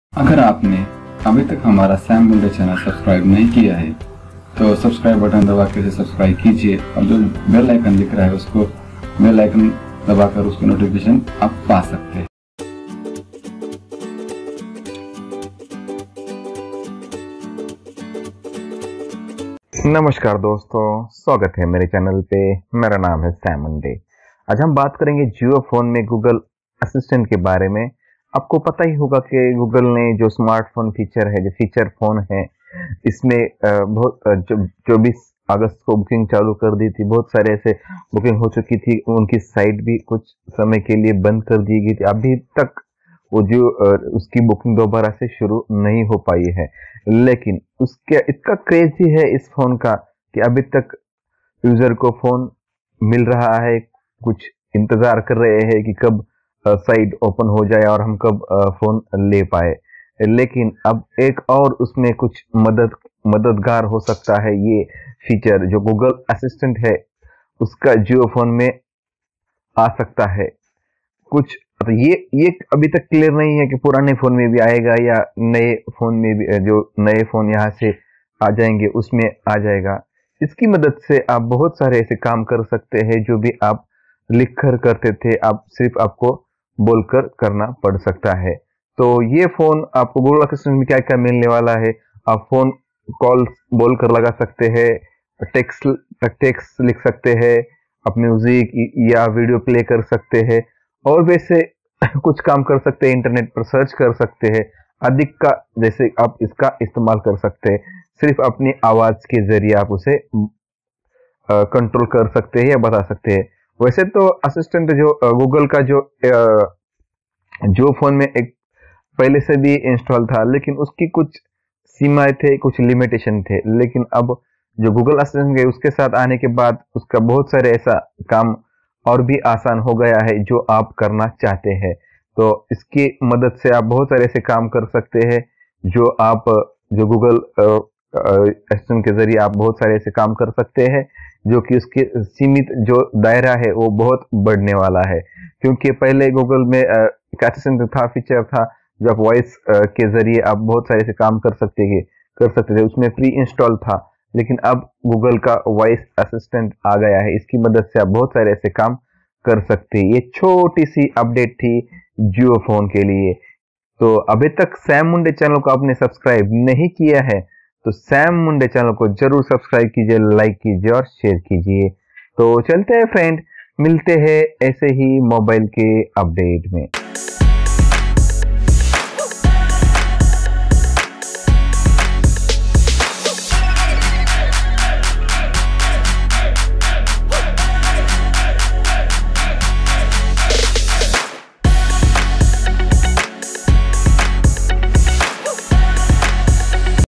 Google Assistant in jio feature phone
best YouTube gear
DSLR camera
lapel mic
hand recorder
USB Condenser Microphone
light stand
LED light

Jio Phone is getting a special version of Google Assistant, marking the first time the Internet giant’s AI-based virtual assistant will come to a feature phone. It will be the second digital assistant for Jio Phone, however, as the 4G VoLTE-capable handset already comes preloaded with an assistant that can take voice commands. The Google Assistant version for Jio Phone will be available in English and Hindi, and respond to voice search queries.

नेशनल पेंशन सिस्टम ( NPS )